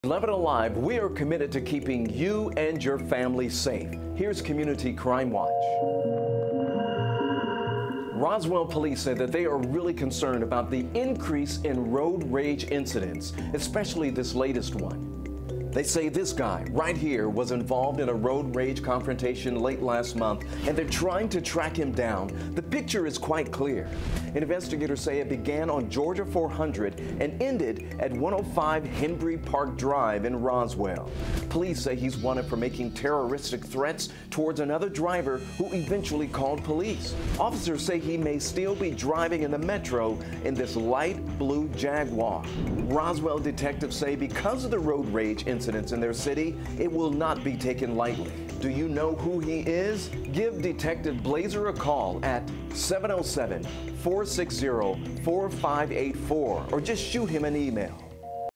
Road rage suspect
Road rage has become a topic of concern in Roswell, especially after this most recent case.